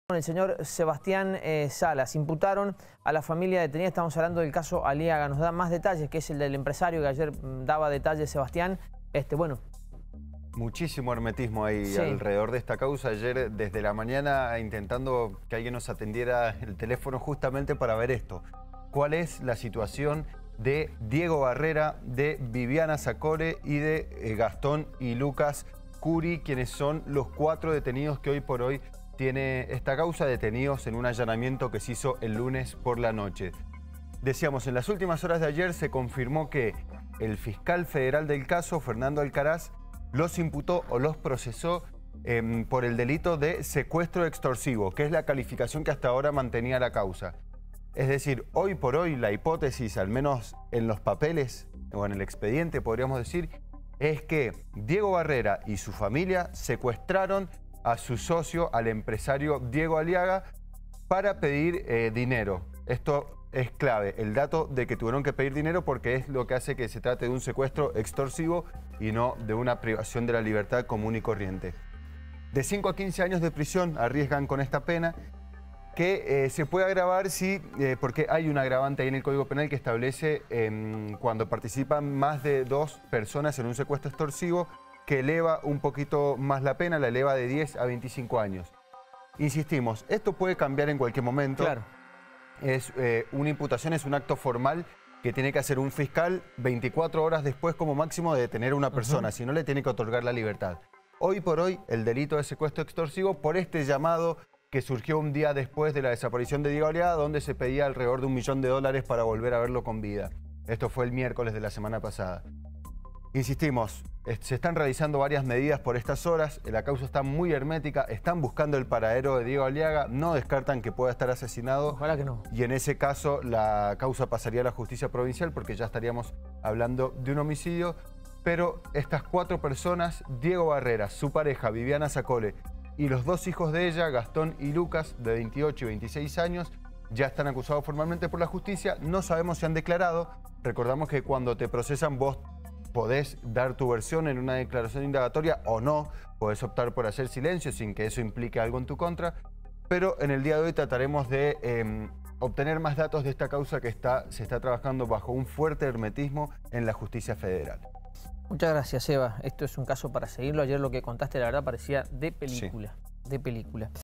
CASO ALIAGA
Caso Aliaga: imputaron a la familia detenida. Diego Barrera, su pareja y dos hijos.